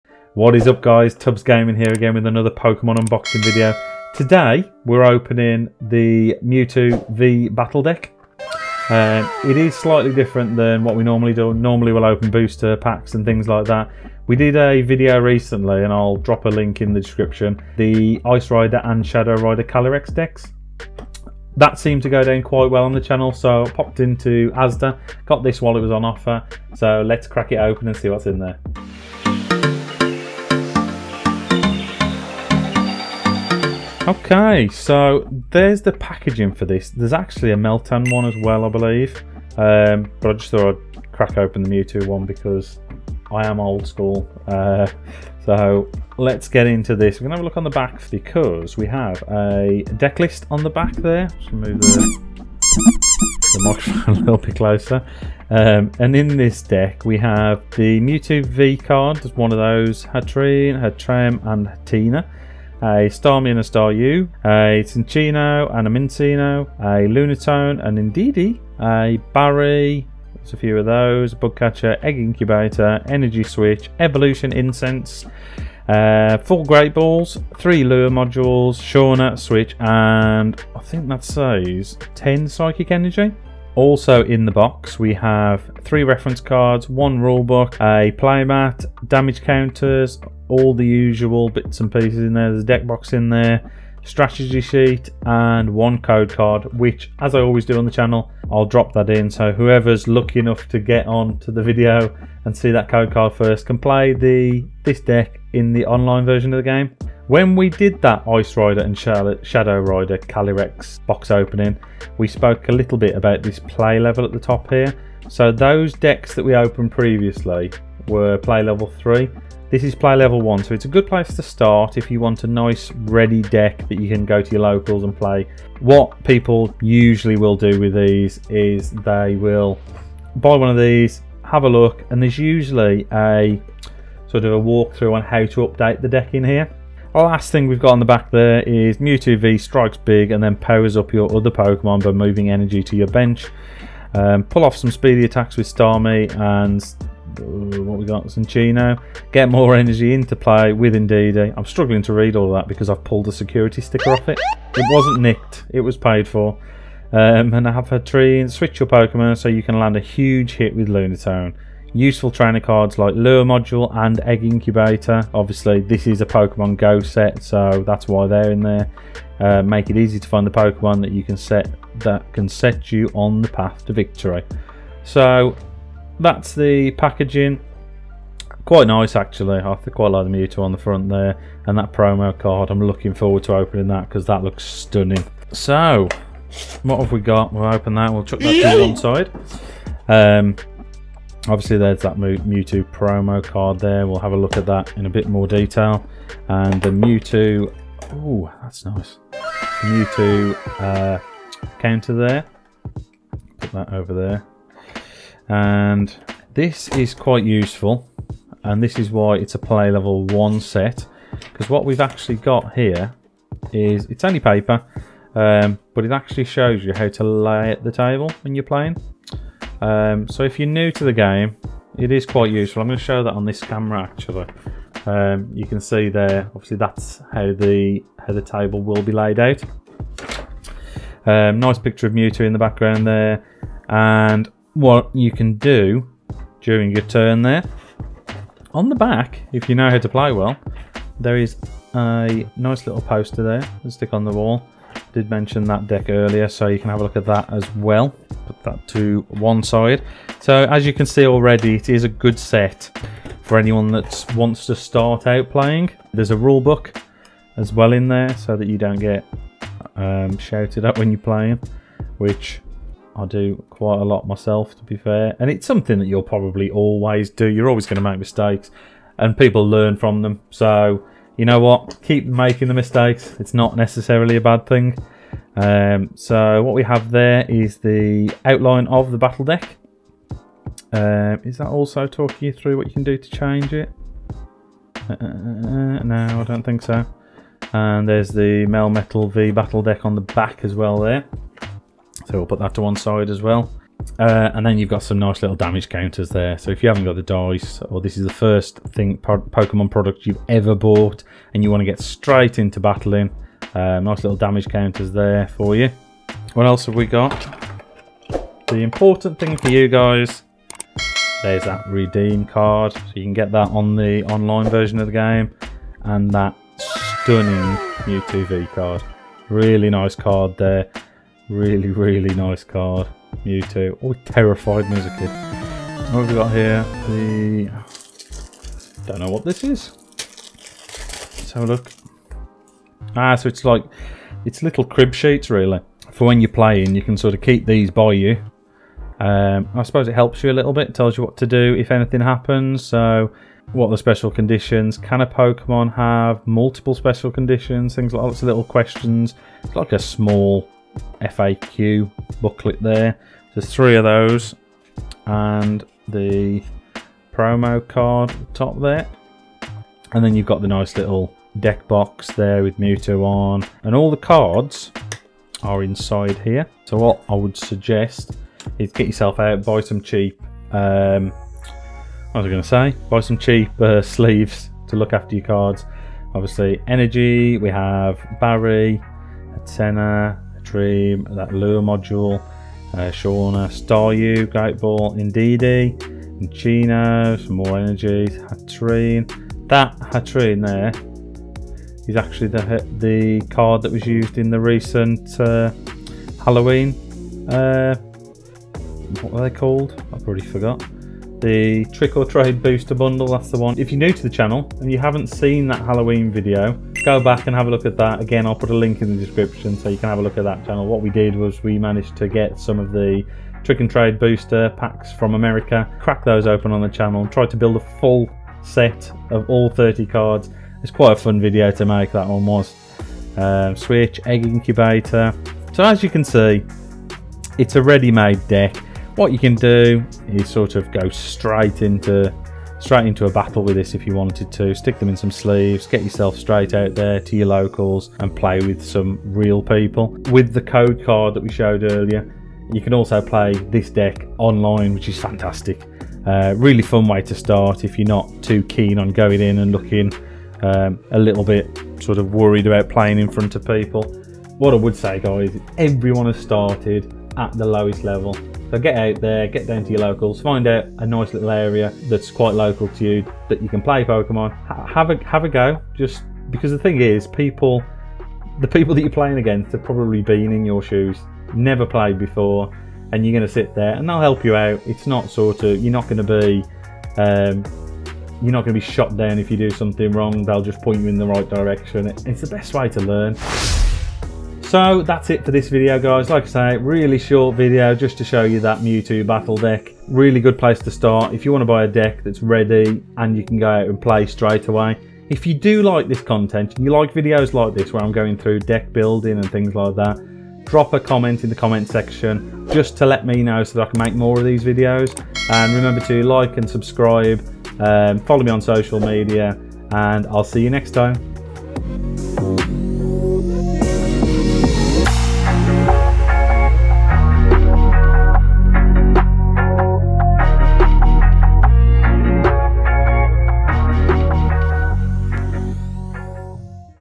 Is The Mewtwo V-Battle Deck WORTH THE MONEY? BEFORE YOU BUY!
Cloaked in Psychic mystery, today we're opening the Mewtwo V-Battle Deck to see what is inside and the ask the question... Is it worth your hard-earned cash?

If you're starting out or thinking about starting to play competitive Pokemon play the Mewtwo battle deck is cheap and has a decent structure but could you spend your money on something else? Will you be able to compete with this deck? How much is it worth? and Where can I buy one?

These are all questions we're going to look at today and give you the advice on whether or not you should pick one up.